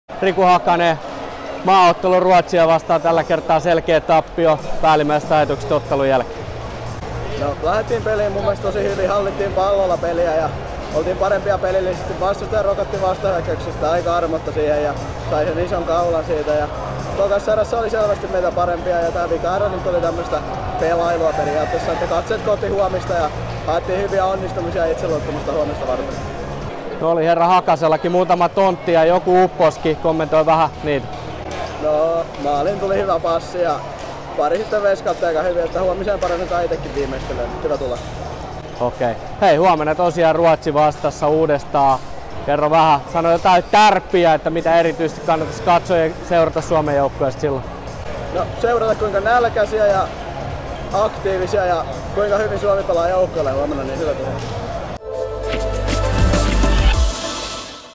SPV-laituri Hakanen Suomen paras Ruotsia vastaan – "Rokottivat armotta vastahyökkäyksistä"
Ruotsin alle 19-vuotiaiden poikien maajoukkue otti vakuuttavan 10–3-voiton Suomesta Finnkampen-tapahtuman ensimmäisessä kohtaamisessa.